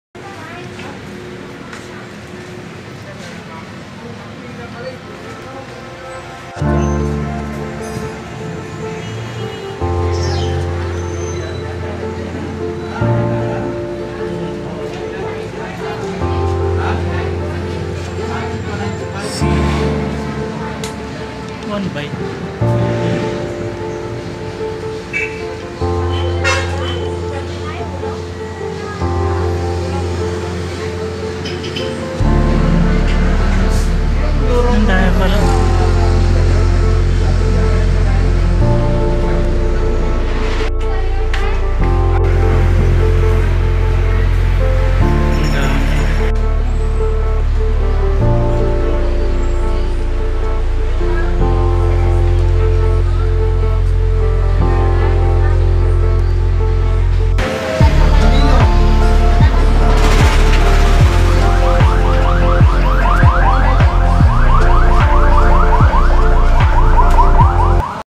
anong mayron sa loob ng fil-star bisycle center imus
a lot to buy in fil-star bicycle center